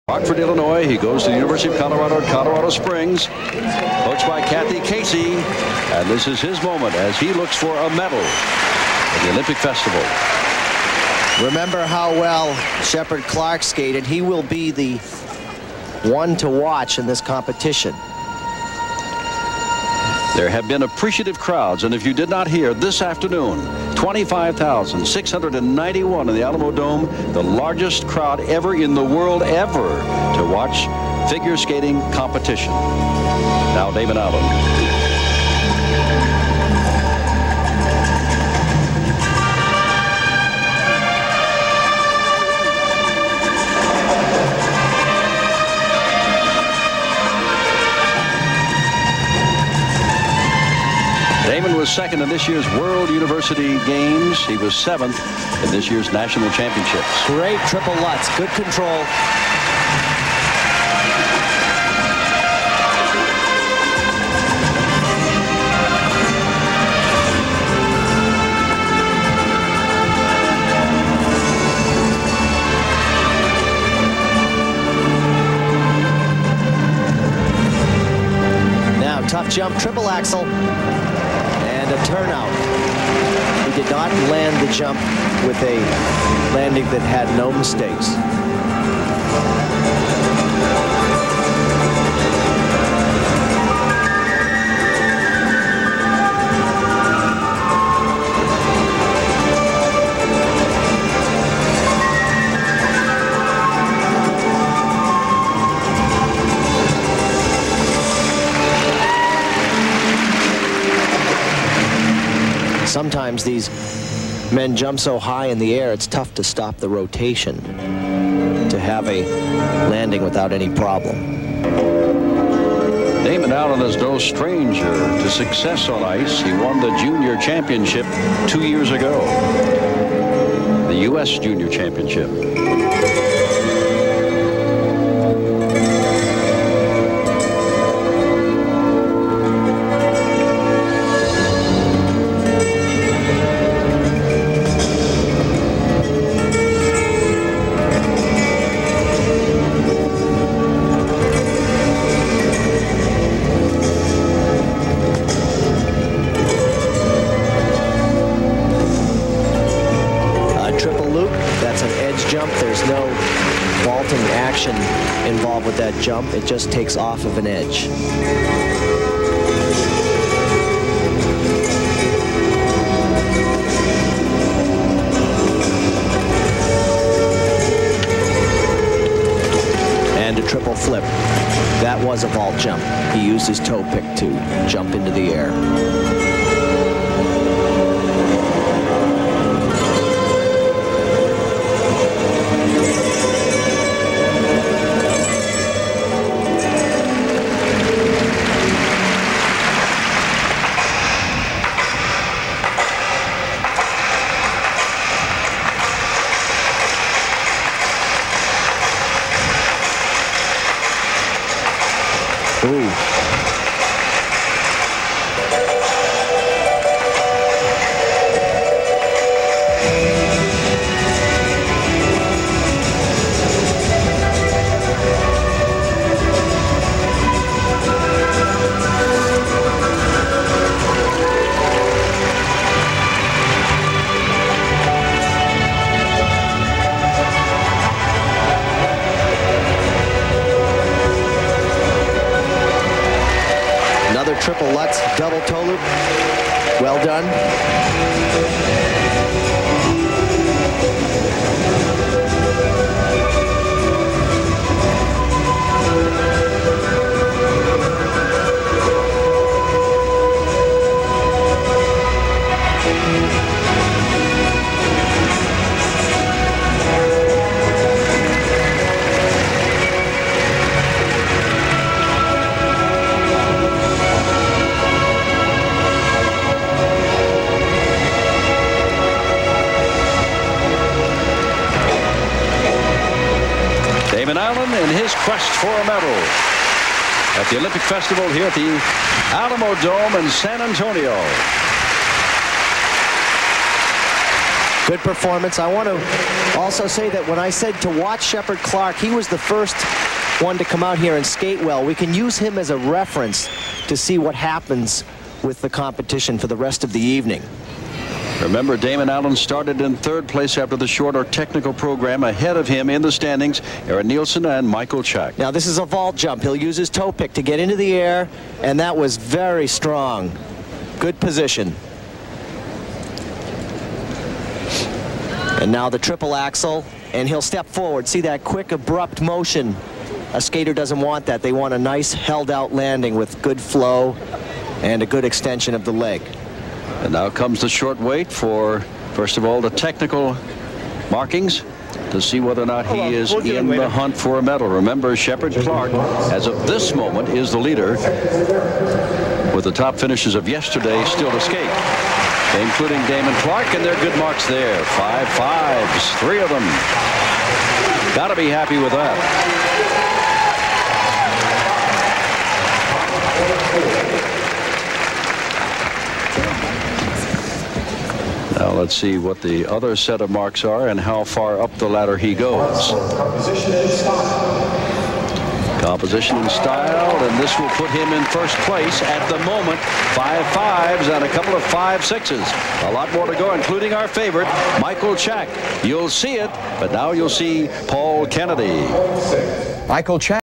Damon Allen - 1993 U.S. Olympic Festival, Men's Free Skate
San Antonio, Texas, USA - 1993 U.S. Olympic Festival, Figure Skating, Men's Free Skate - Damon Allen